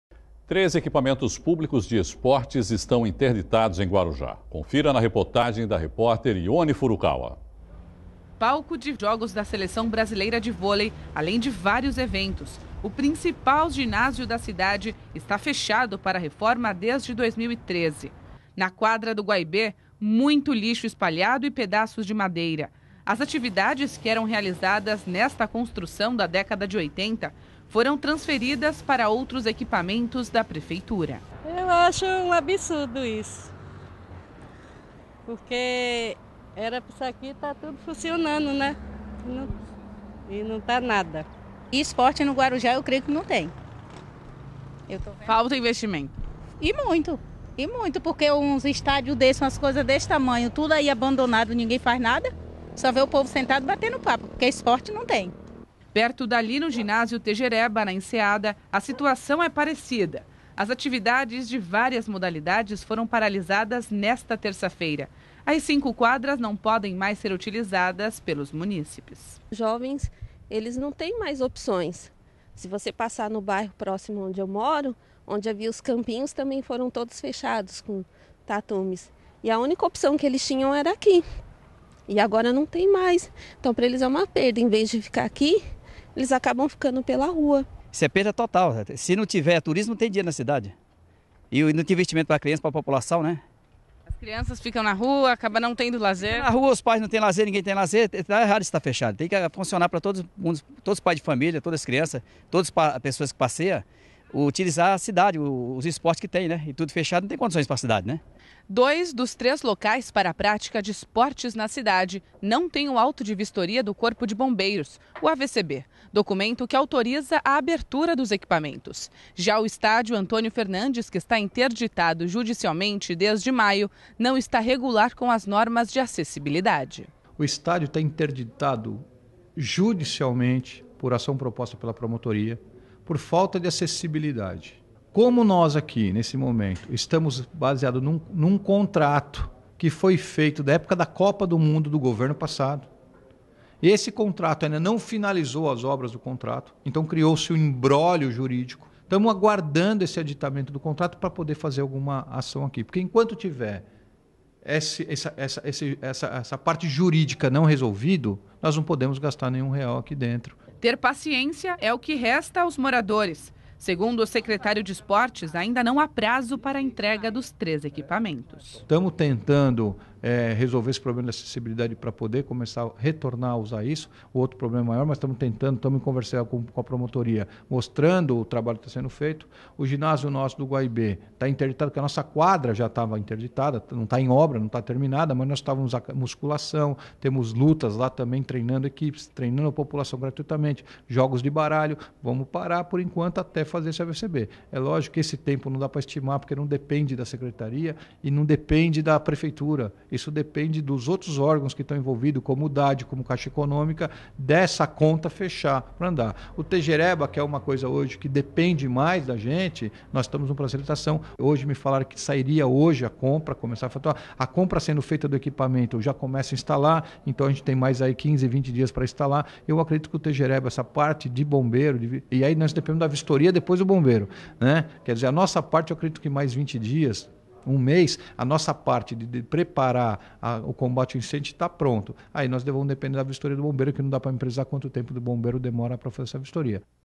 Três equipamento públicos de esportes estão interditados no Guarujá
Dois dos três locais não tem o Alto de Vistoria do Corpo de Bombeiros. Já o terceiro não está regular com as normas de acessibilidade.